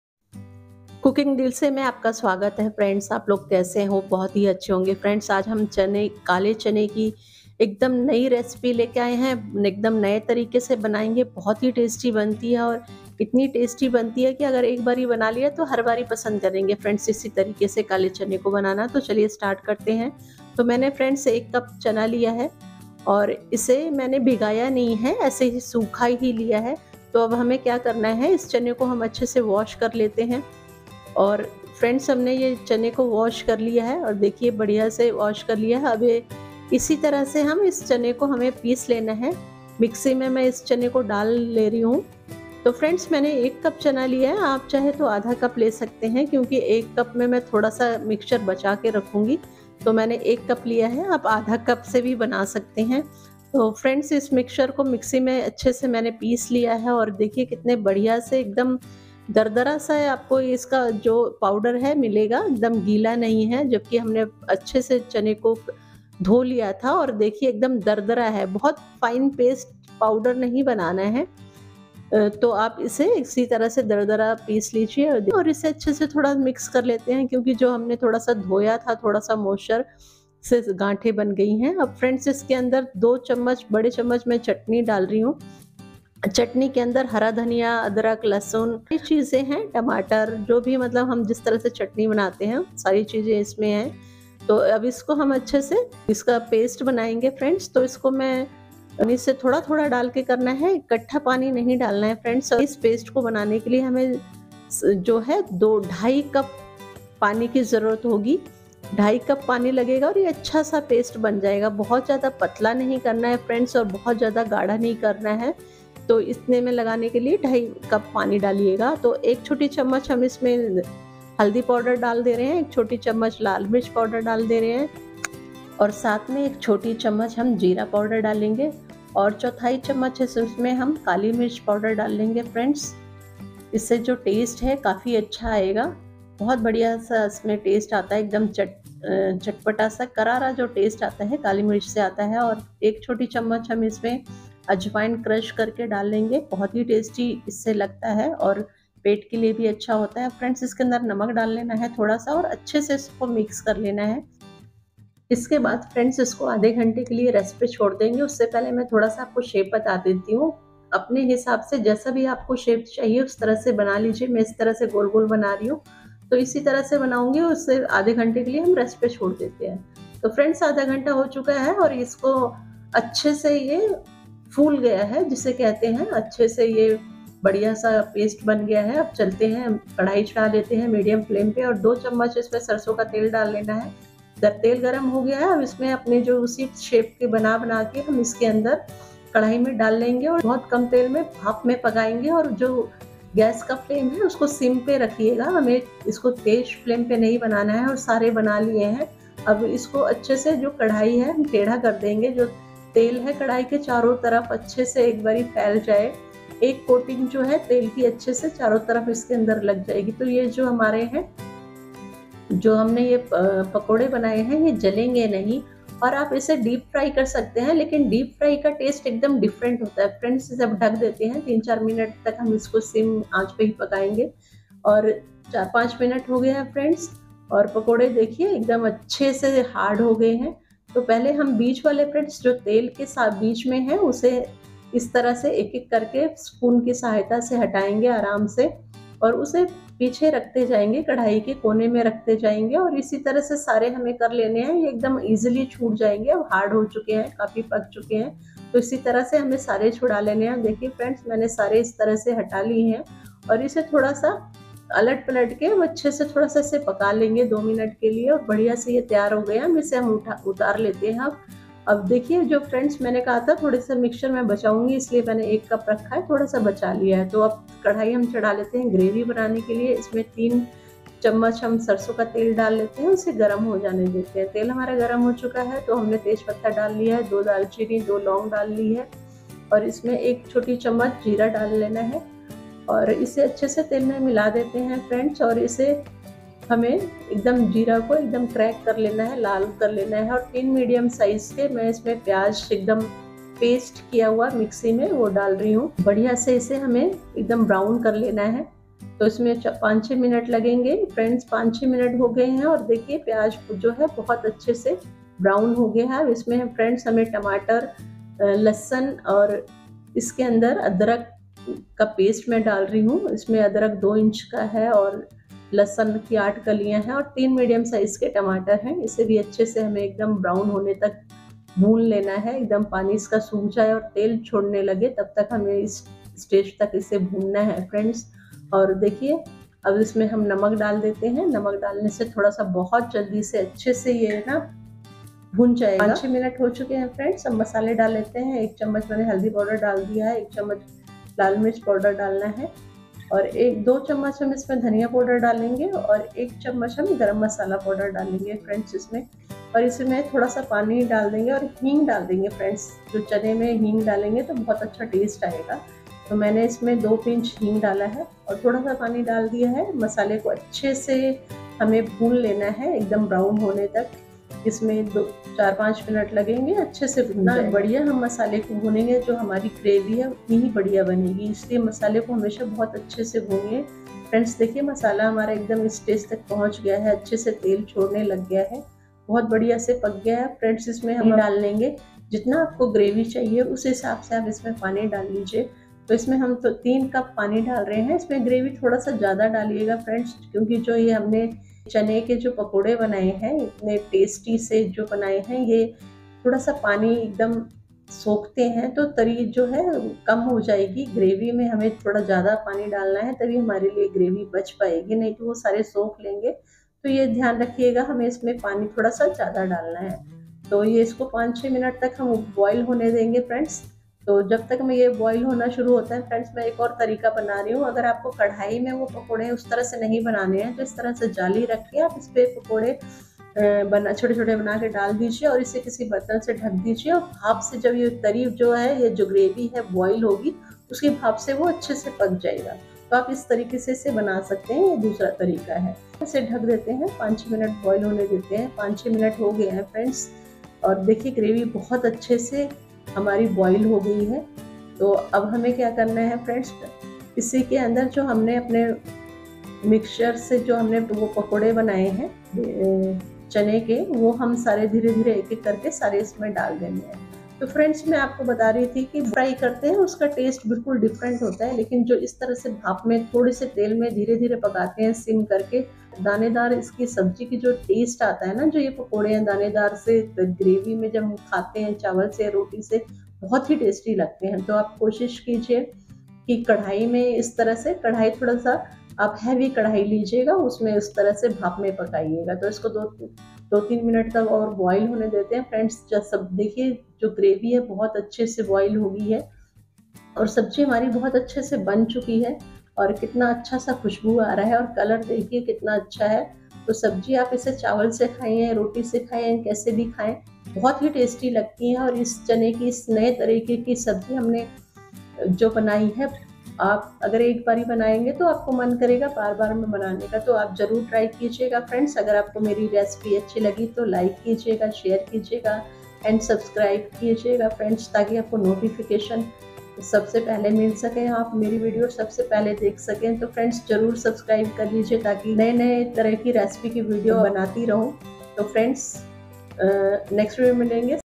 काले चने की नई मजेदार रेसिपी बनाए एकदम नए तरीके से स्वाद ऐसा कि भूल नहीं पाएंगे आप|kala chana recipe
Kale chane ki ekadam nai recipe@CookingDilSe2022 ,kala chana curry recipe,Kale chane ki sabji kaise banaen,kala chana curry recipe,
serving 6 to 7 persons
ingredients:-
For black gram mixture:-
one cup black gram
one teaspoon turmeric powder
one teaspoon red chilli powder
one teaspoon jeera powder
1/4th teaspoon black pepper seeds powder
one teaspoon ajwain seeds
half teaspoon salt
For Gravy:-
3 tablespoon mustard oil
2 bay leaves
too small pieces of cinnamon
2 cloves
one teaspoon cumin seed
3 medium size onion
3 medium size tomato 🍅🍅🍅
2 inch piece of Ginger
8 cloves of garlic
taste to Salt
two teaspoon coriander powder
one teaspoon garam masala powder
to pinch asafoetida